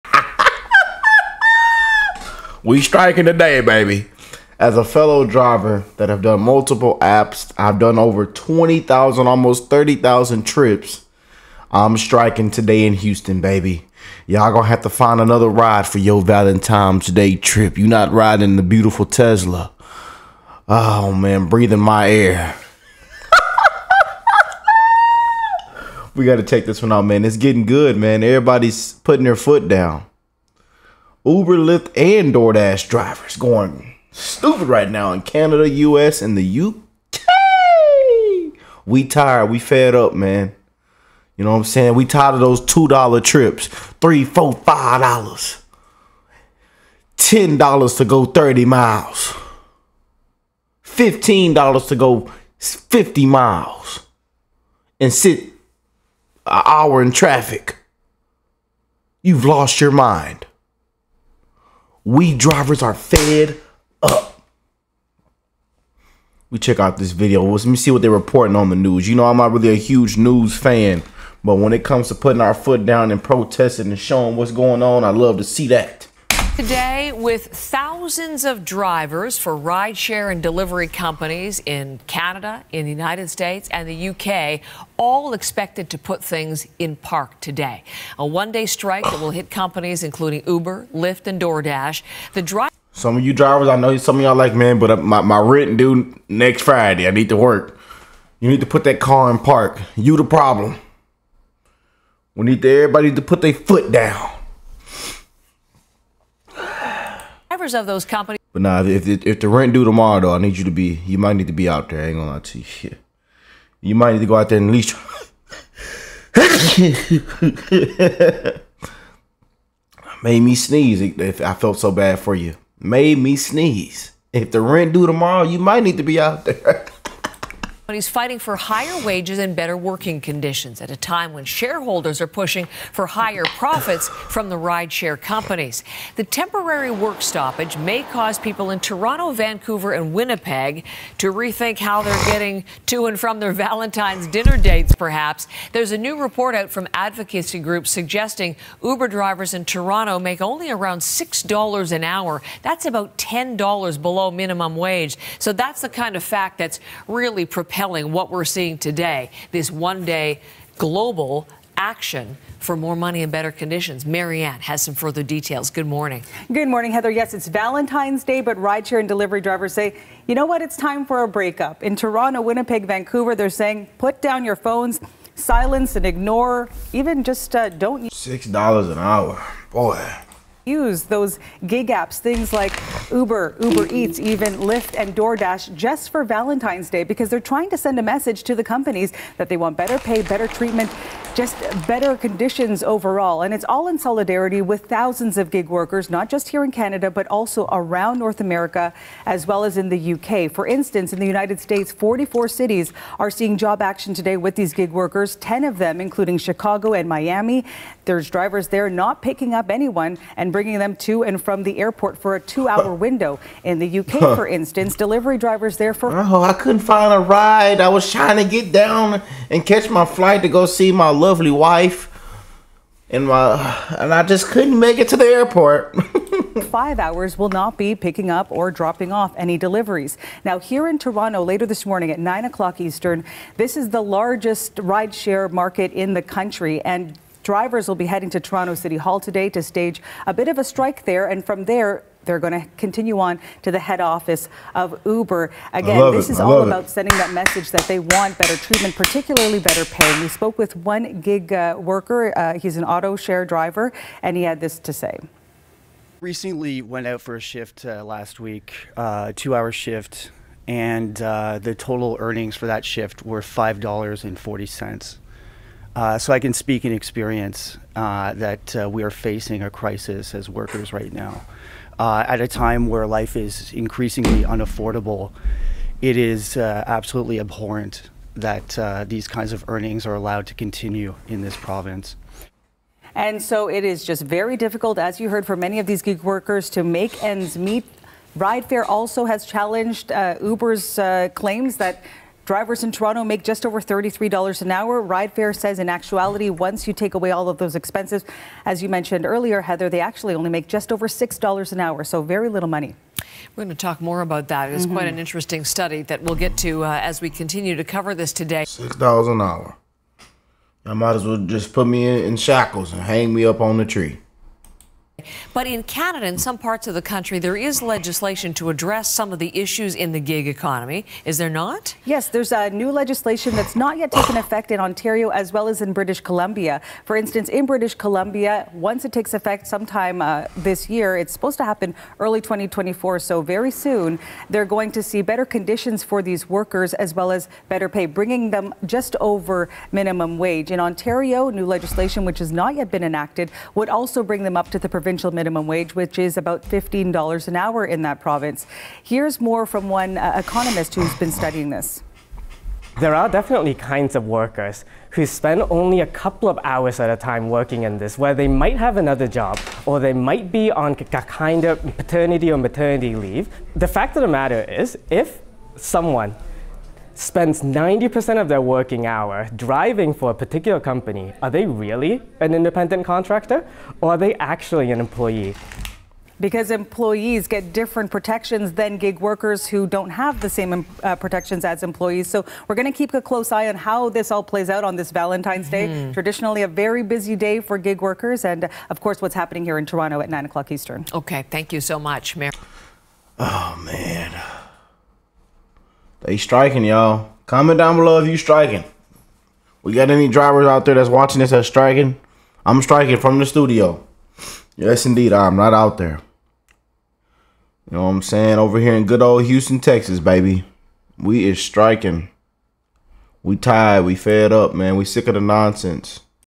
Uber, Lift, and DoorDash drivers striking in US, Canada, and U.K.
Drivers from ride-hailing and delivery app companies like Uber, Lift, and DoorDash are going on strike for one day. Workers are seeking higher wages and better working conditio

Im in houston tx uber ridesharing here to help & educate and spread awareness